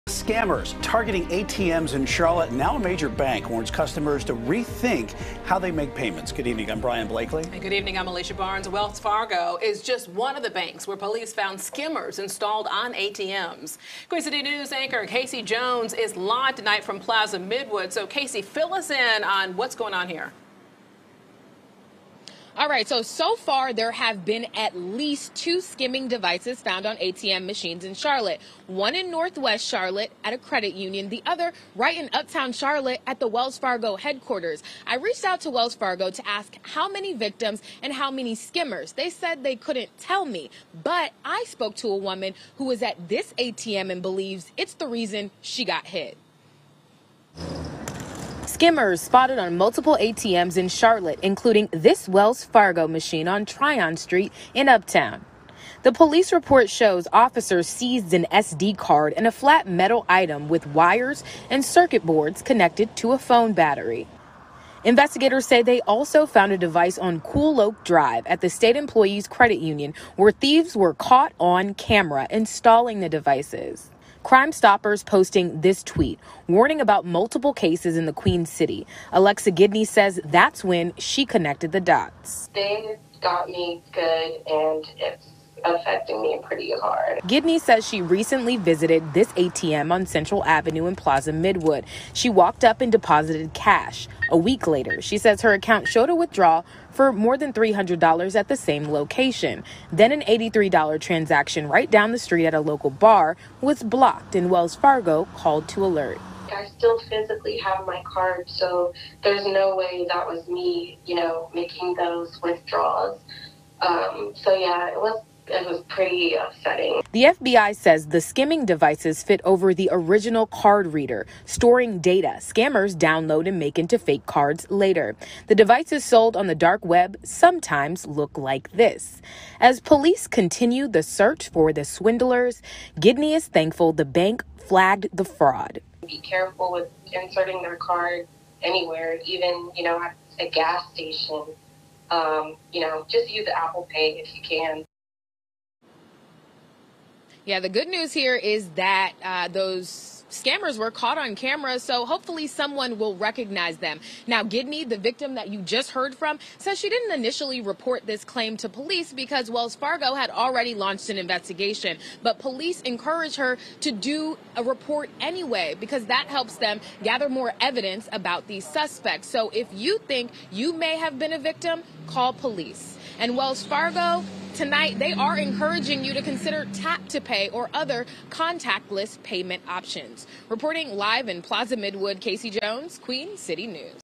Skimmers found on Charlotte ATMs
Investigators say they also found a device on Couloak Drive at the State Employees Credit Union, where thieves were caught on camera installing the devices.